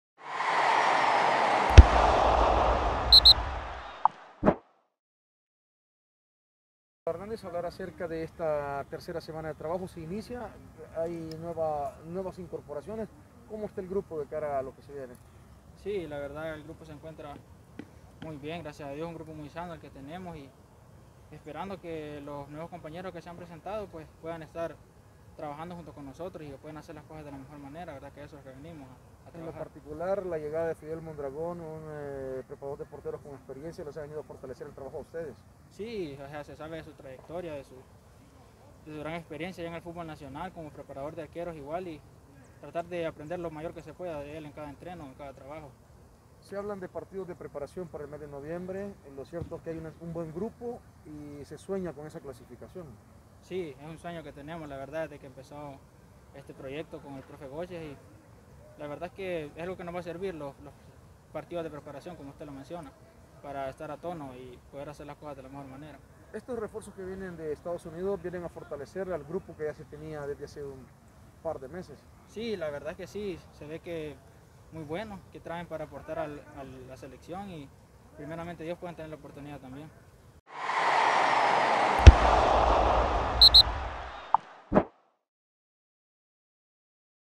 Declaraciones Danilo Hernández, Preseleccionado Nacional Sub20
Es un sueño que tenemos desde que iniciamos este proyecto y queremos darle la clasificación a nuestro país.